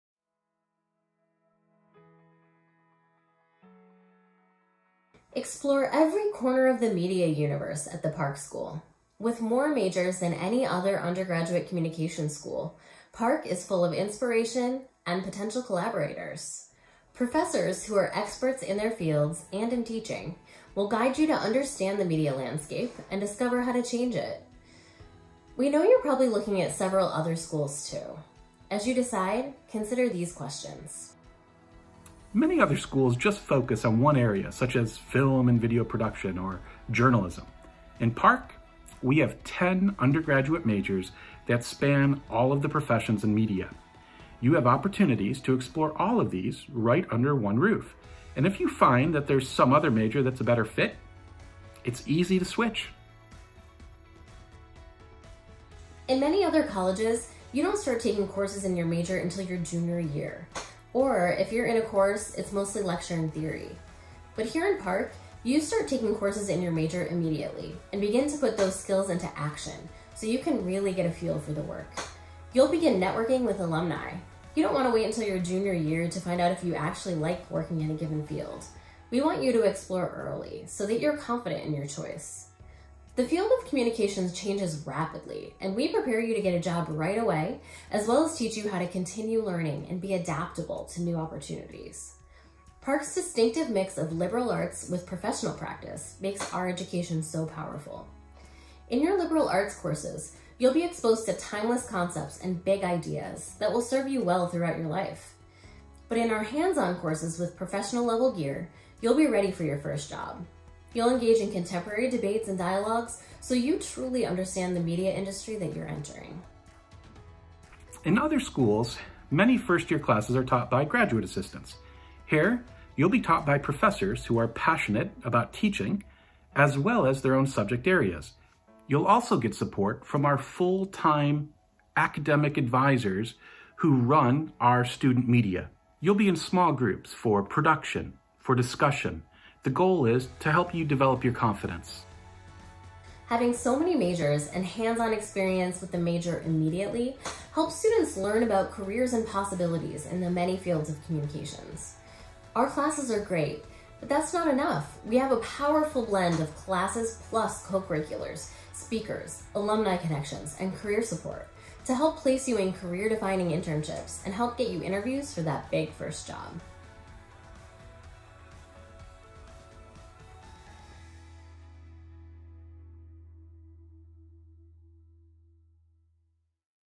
As You Decide - Some Questions Answered
As you decide, these faculty members will help answer some questions on majors, hands-on work, networking with alumni, career development, advisors, as well as combining coursework with co-curriculars to create a tailored experience.

This is a student-produced video created by the co-curricular Park Productions. Even while quarantined, students and faculty maintain high spirits, collaborate remotely, and pull together available resources to bring to you their love of Park.